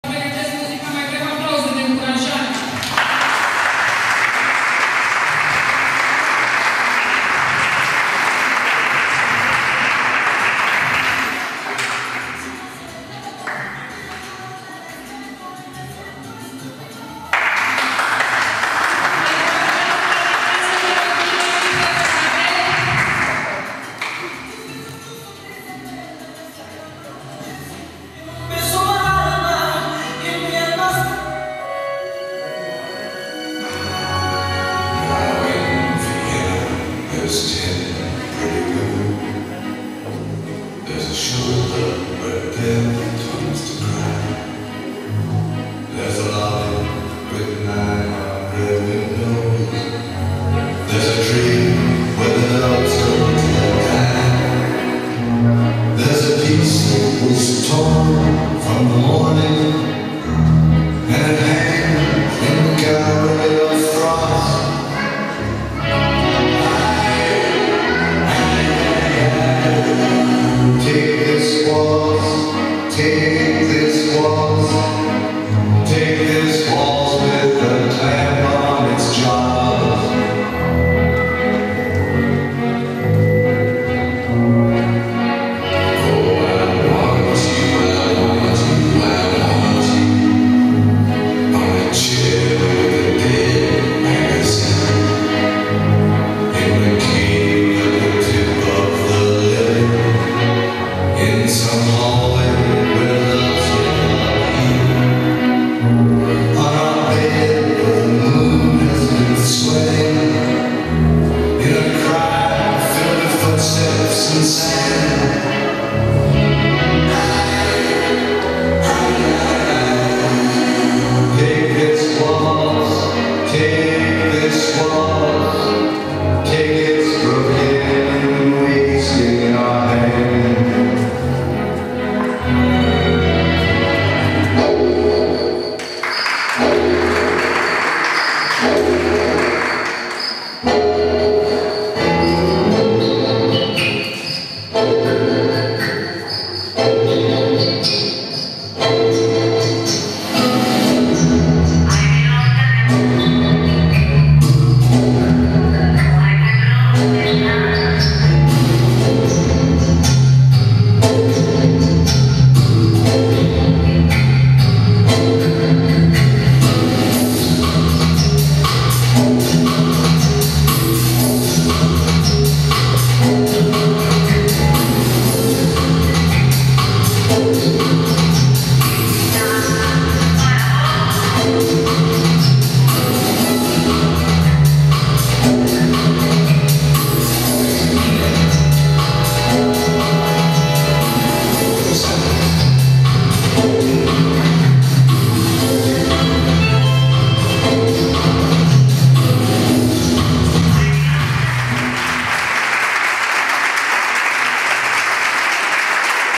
Andrei&Ana- Vals& Tango
Dupa cateva sedinte particulare pentru nunta la scoala Feel the Rhythm,  ne-am hotarat sa facem si o mica demonstratie de dans la deschiderea oficiala a cursurilor de dans desfasurate la "Rainbow" Dance Studio